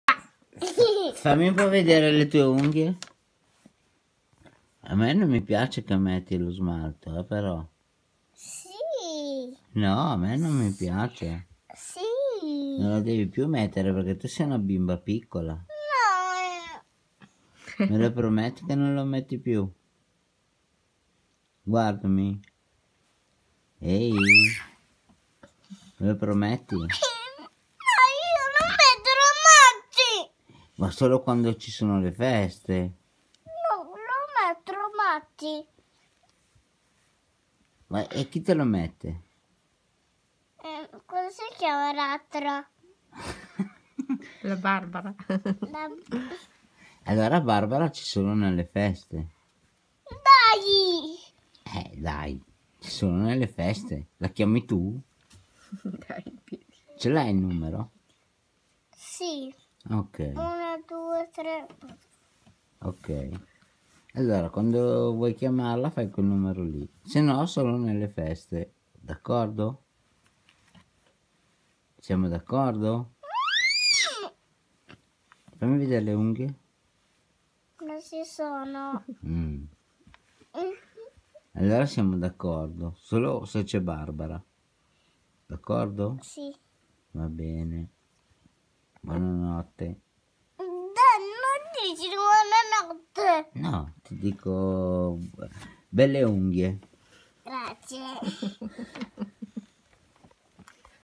Camilla con lo smalto
Discussione tra papà e figlia